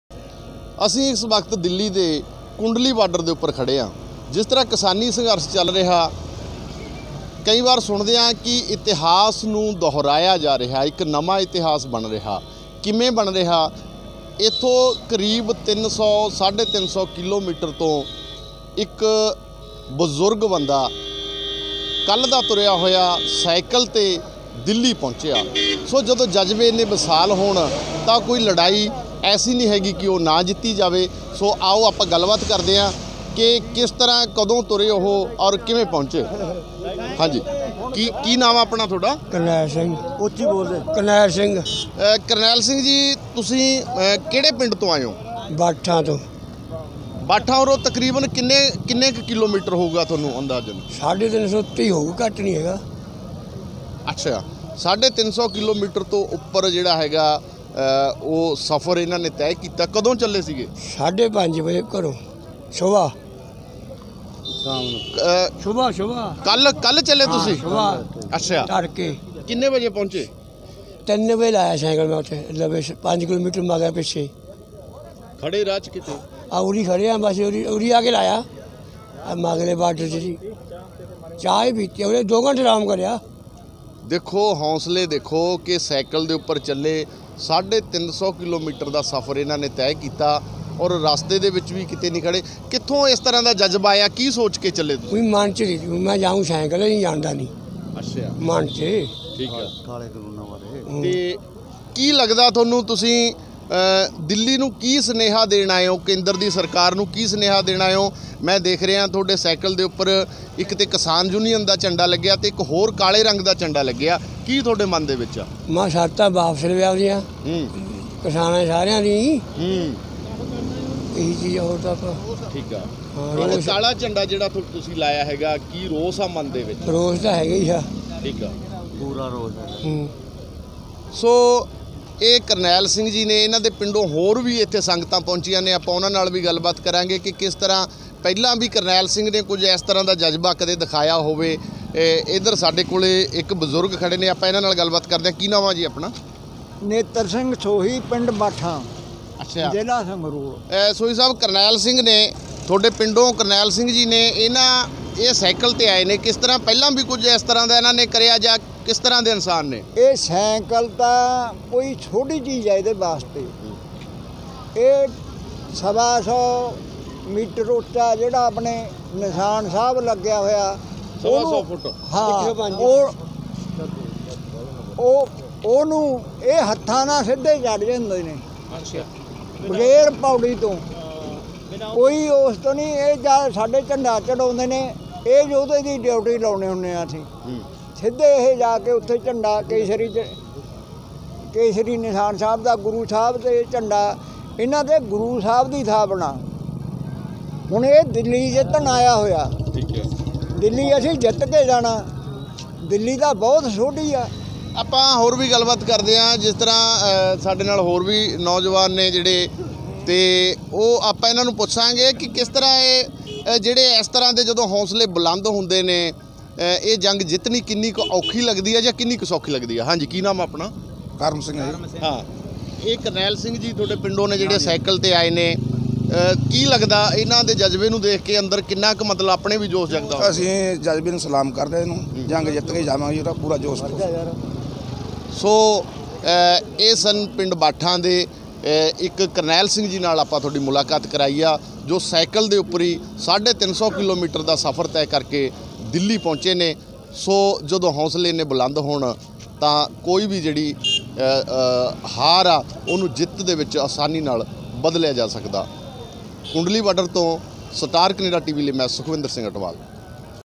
Punjab News : ਇਹ ਵੀਡੀਓ ਦੇਖ ਉੱਡਜੂ ਵੱਡੇ ਮੰਤਰੀਆਂ ਦੀ ਨੀਂਦ | Delhi Kundli Border News | Star Canada Tv
ਇਹ ਵੀਡੀਓ ਦੇਖ ਉੱਡਜੂ  ਵੱਡੇ ਮੰਤਰੀਆਂ ਦੀ ਨੀਂਦ
70 ਸਾਲ ਦੇ ਬਾਬੇ ਨੇ ਚੱਕਿਆ ਸਾਇਕਲ ਤੇ ਪਹੁੰਚ ਗਿਆ ਧਰਨੇ ‘ਤੇ
ਹੁਣ ਤਾਂ ਮੰਤਰੀ ਕੱਢ ਦੇਣ ਦਿਲਾਂ ‘ਚੋਂ ਭੁਲੇਖੇ ਕਿ ਇਹ ਪਿੱਛੇ ਹੱਟ ਜਾਣਗੇ